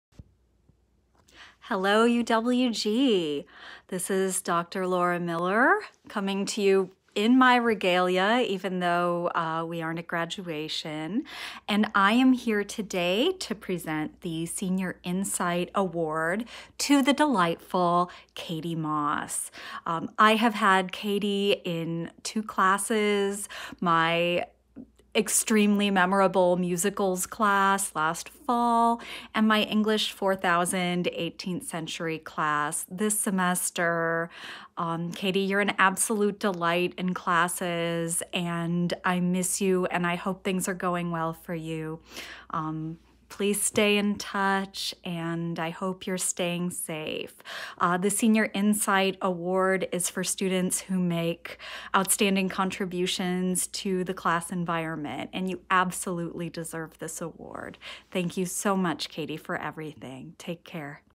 Senior Insight Award: Katie Moss
Dr. Laura Miller congratulates Katie Moss, recipient of the Senior Insight Award